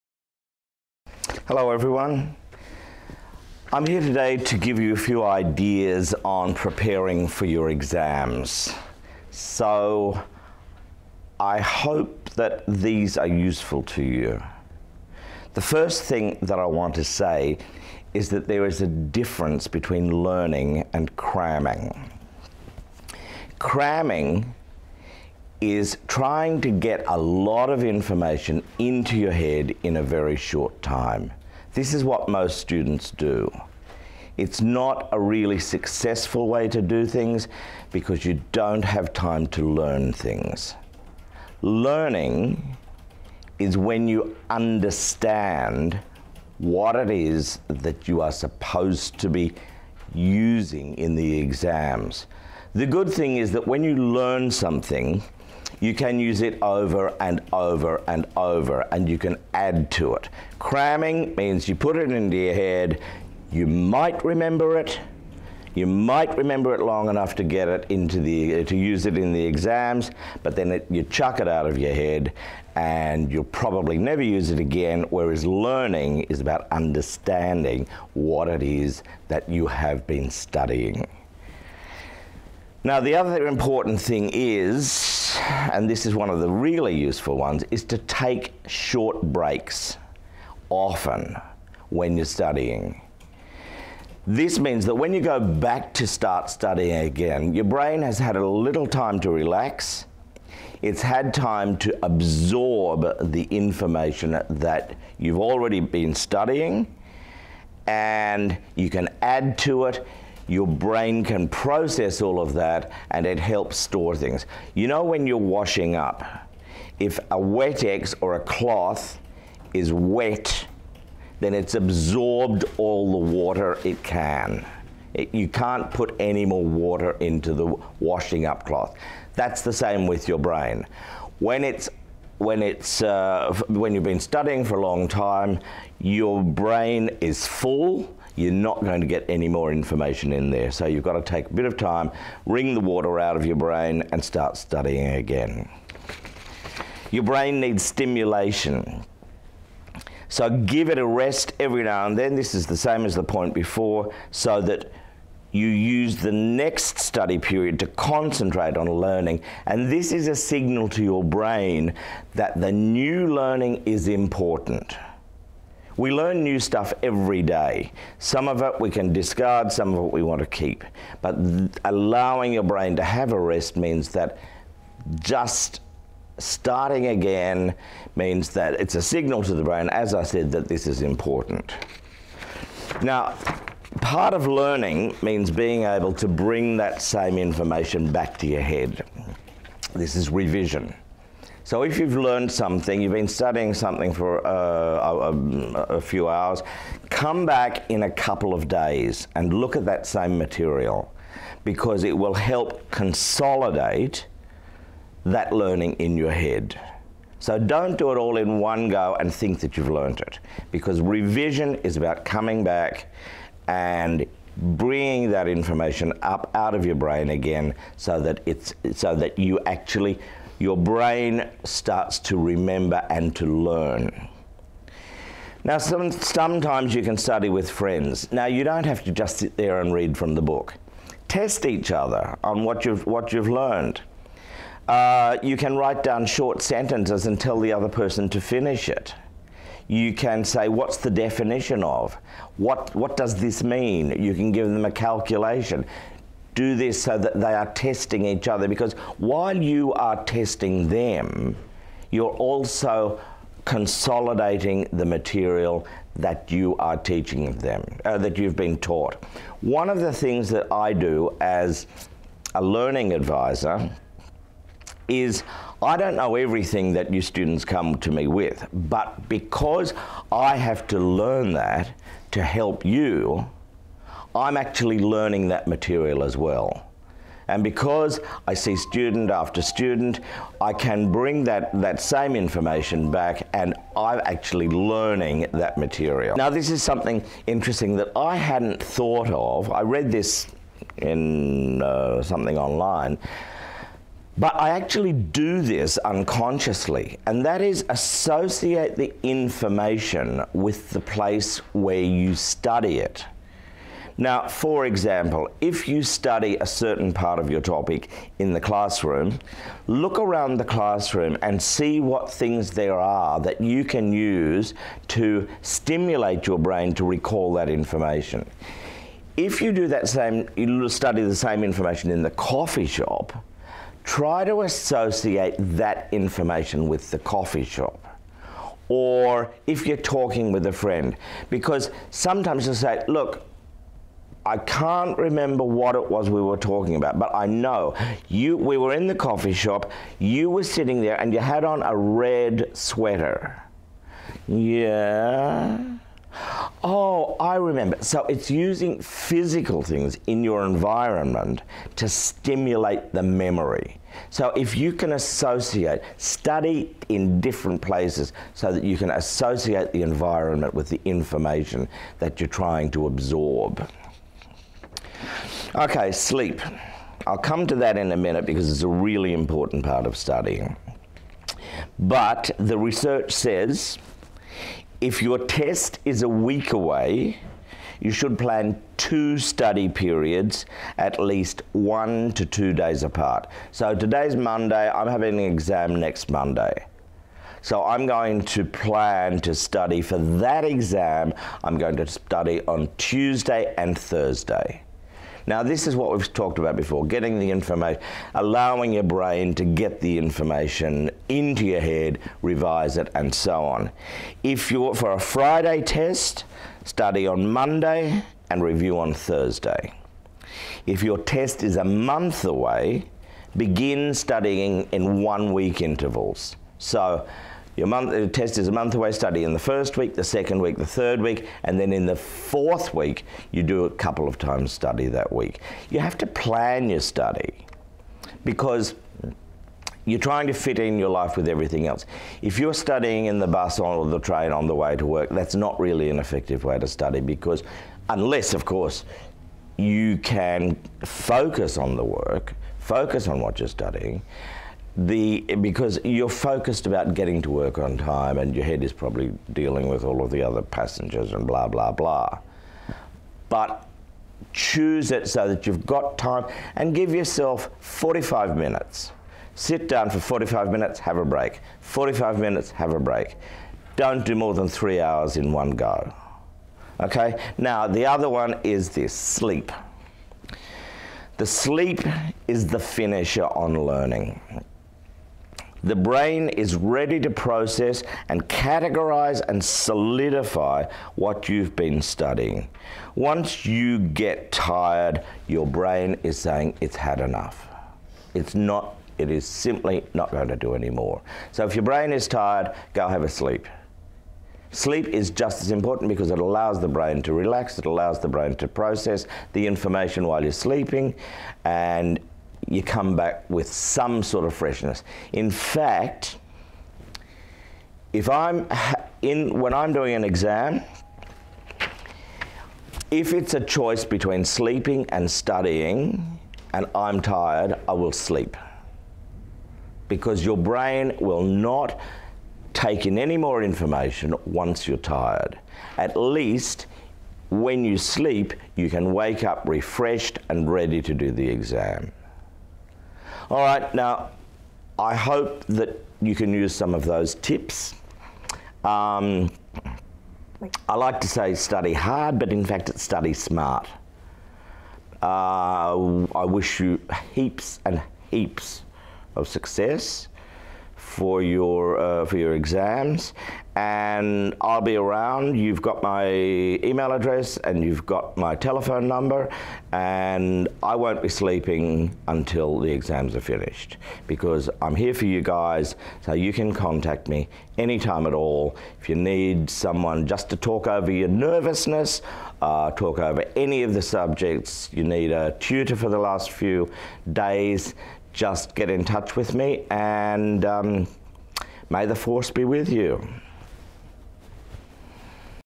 VU Sydney Learning Support Adviser | Exams Preparation Tips
Victoria University Sydney has a dedicated Learning Support Advisor: Mr Ian Steep. All VU Sydney students can find him in Learning Commons on Level 3.

At the Exam Preparation workshops, he can help the students to improve their learning for great exam results. Ian is available from Mondays to Friday – 9.30am to 4.00pm.